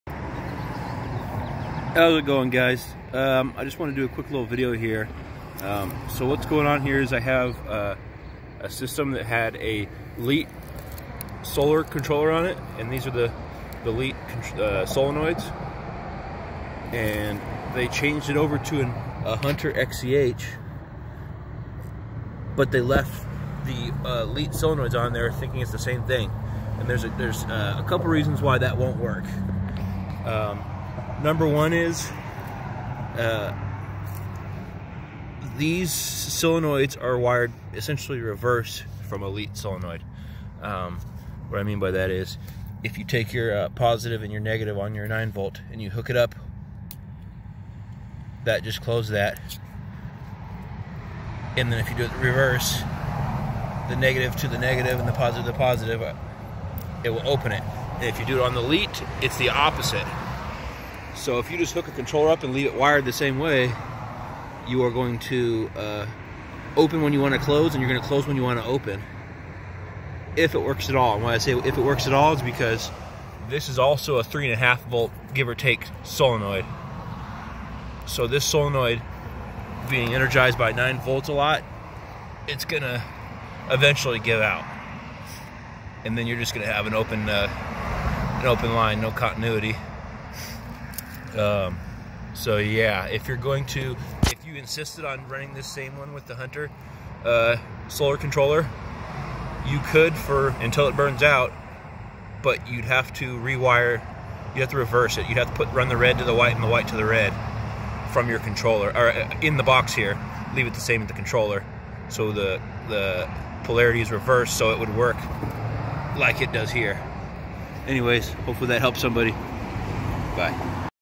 LEIT Solenoids VS Rainbird DC Latching Solenoids
In this video I briefly explain a common problem I see in the field a lot. An irrigator will attempt to use the existing Leit Solenoids after upgrading the controller to a 9V solar timer such as a Hunter XCH. These solenoids are not cross compatible as I explained in the video.